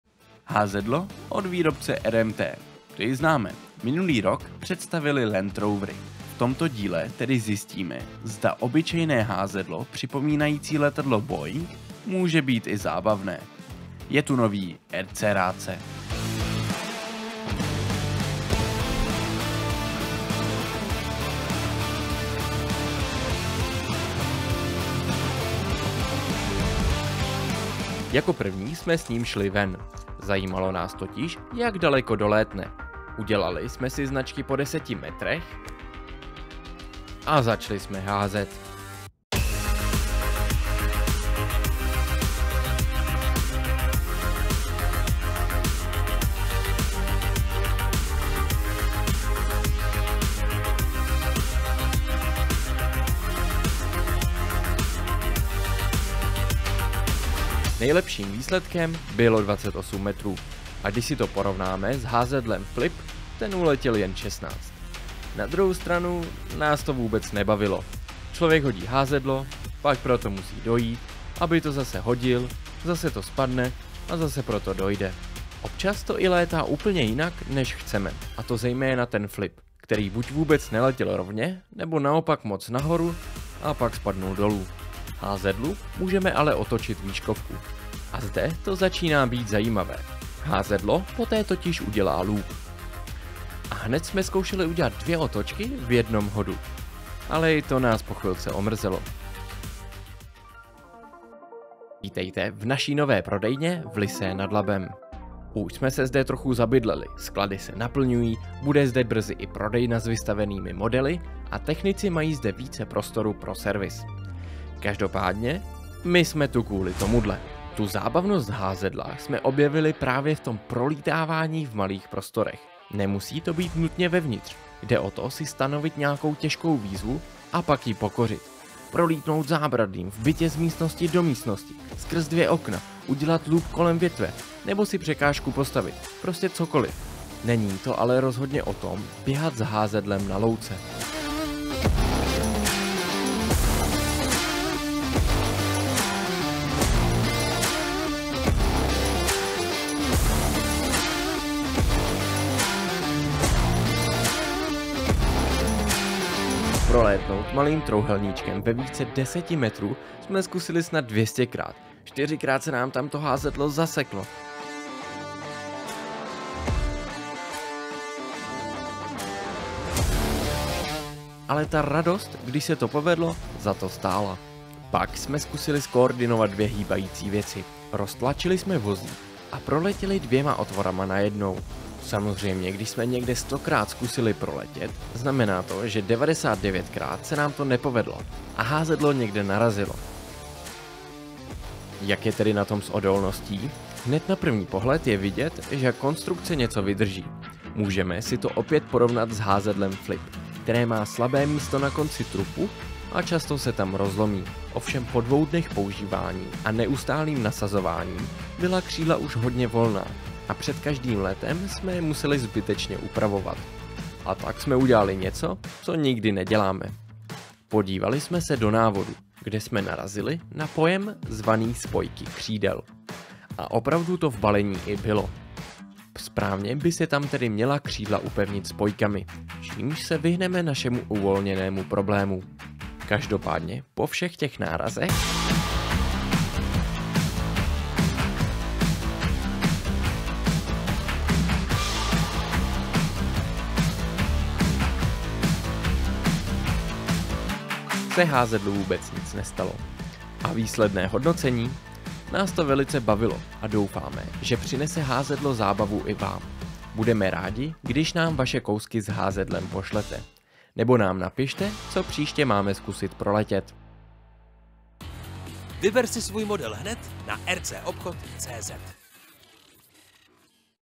RC-rádce // Házedlo RMT Longfly Glider // RCobchod.cz
Házedlo RMT long fly glider je házedlo pro děti od výrobce RMT. Tuto firmu už od nás znáte. Minulý rok představili skvělé Land rovery. Letos tu mají další novinky a tato je jedna z nich. Házedlo je designováno jako dopravní letadlo Boeing 737.